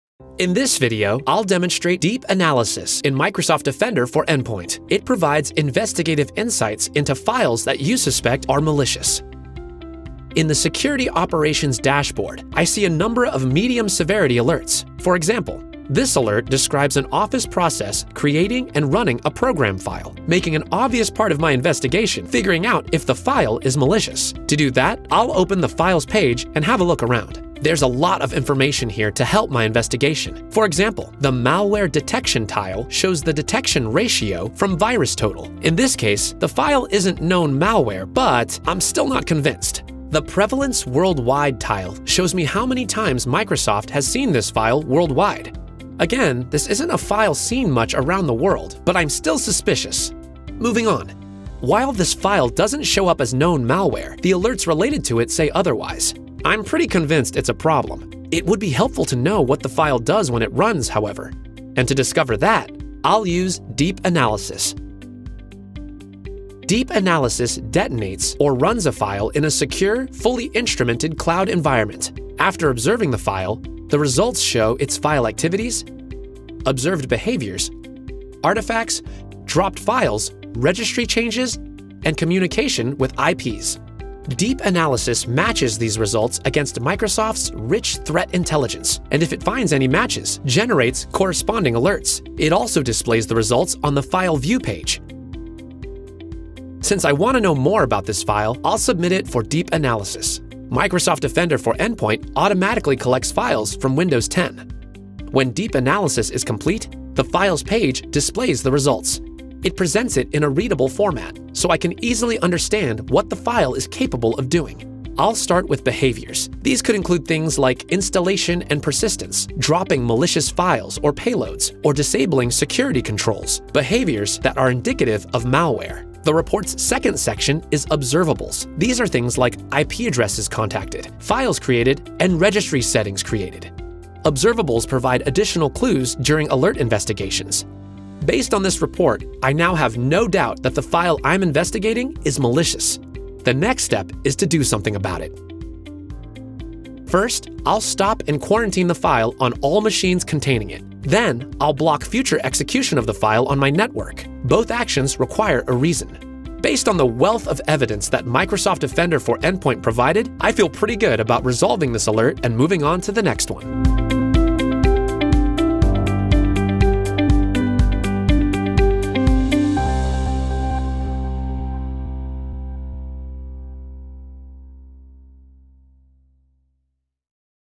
Deep analysis feature in Microsoft Defender for Endpoint
The deep analysis feature executes a file in a secure, fully instrumented cloud environment. Deep analysis results show the file's activities, observed behaviors, and associated artifacts, such as dropped files, registry modifications, and communication with IPs. Deep analysis currently supports extensive analysis of portable executable (PE) files (including .exe and .dll files).

The deep analysis summary includes a list of observed behaviors, some of which can indicate malicious activity, and observables, including contacted IPs and files created on the disk. If nothing was found, these sections will simply display a brief message.

Results of deep analysis are matched against threat intelligence and any matches will generate appropriate alerts.